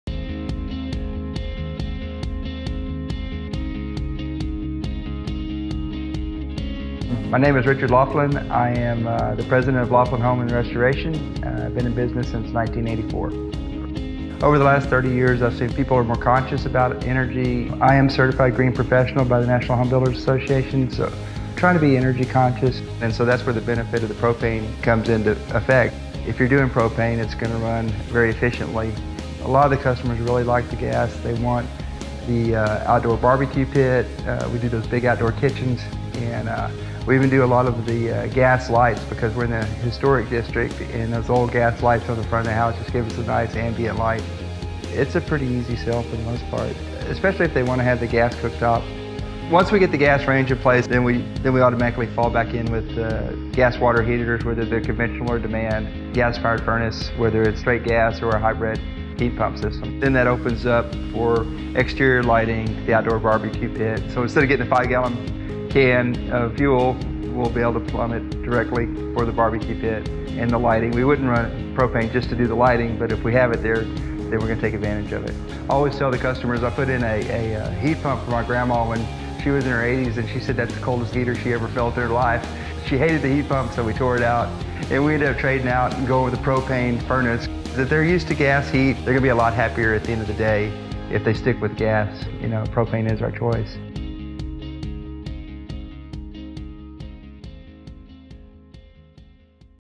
Propane Residential Testimonial: Laughlin Homes and Restoration
Laughlin Homes & Restoration has been in business since 1984. During the past 30 years, Richard Laughlin has seen people grow more conscious about energy. Laughlin is a certified green professional through the National Homebuilders Association, and propane plays a significant role in the types of homes he builds and restores. Starting with the gas cooktop and expanding outward, Laughlin includes propane systems in space and water heating, outdoor living, and even exterior lighting, where it adds desirable ambiance, particularly in historic districts. Laughlin takes advantage of propane’s full range of residential applications to enhance overall customer satisfaction.